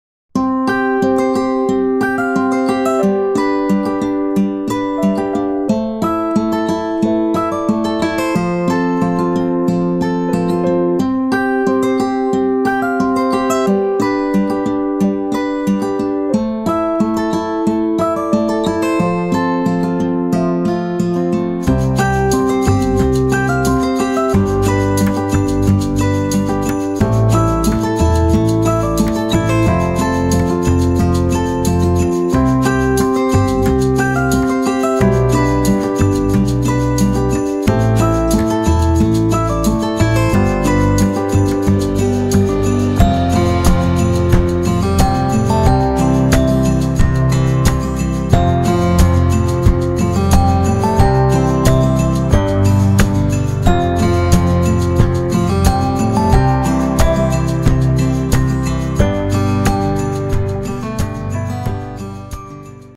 How to Use Repetition for Cohesive Garden Design
These are a couple ideas I use for keeping  garden plantings pleasing and cohesive.